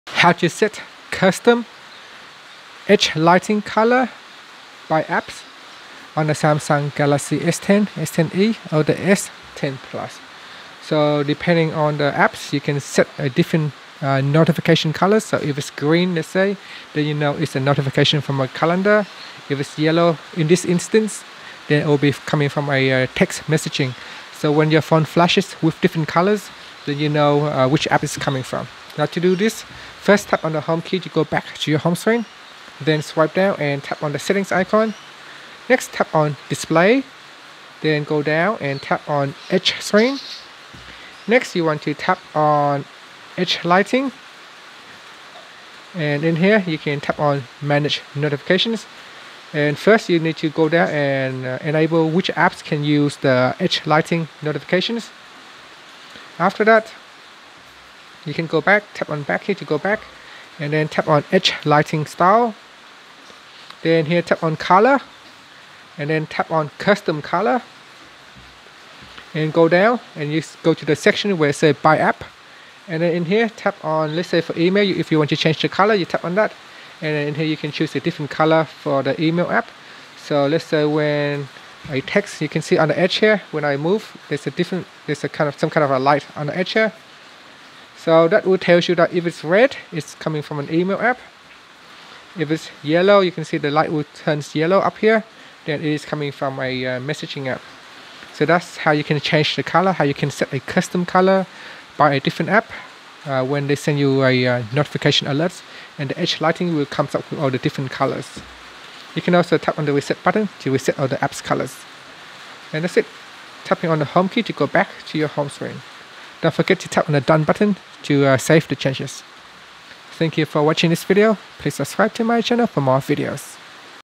Galaxy S10 / S10+: Set Custom Edge Lighting Color By App
Learn how you can set custom Edge Lighting color by app on Galaxy S10 / S10+.